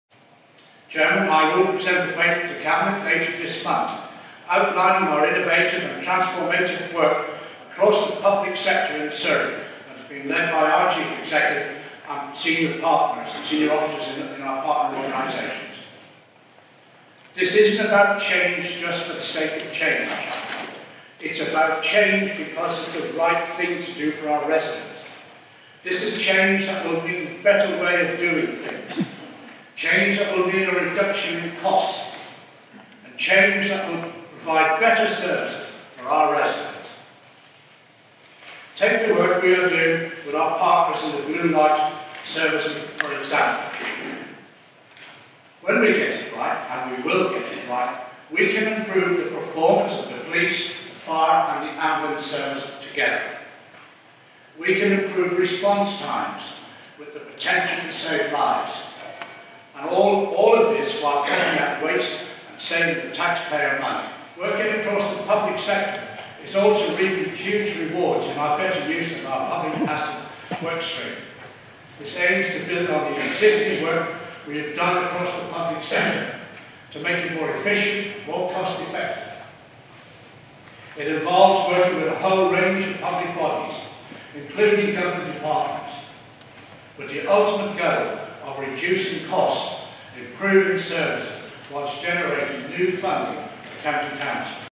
Surrey leader on transforming public services
David Hodge outlines long-term plans for closer working between the county council and emergency services. The proposals will go to the council's Cabinet on 22 October.